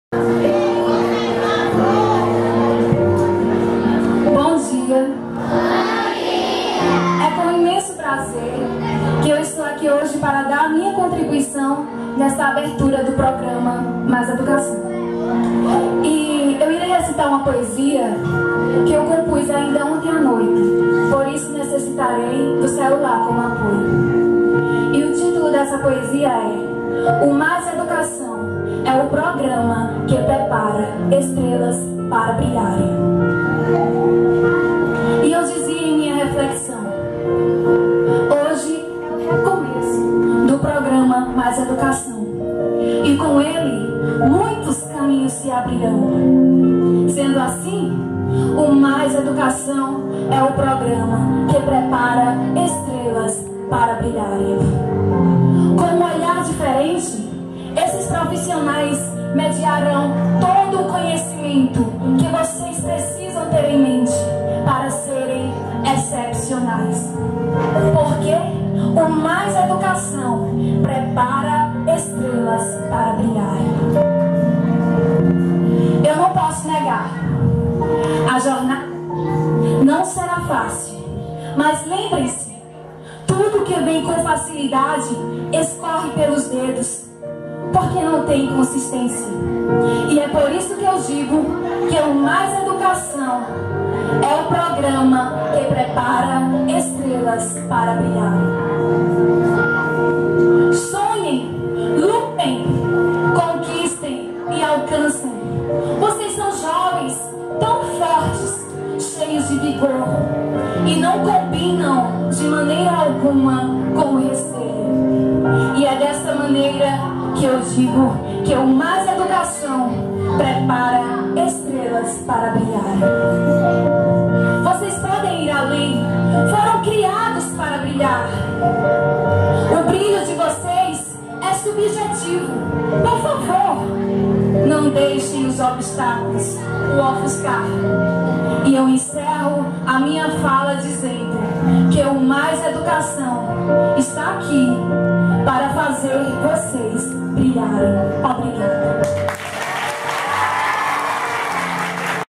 Professora Adriely Lopes 08\05\2018.
Barra do Tarrachil-BA. Recepção dos Alunos do novo Mais educação da escola municipal B.F.S. 2018.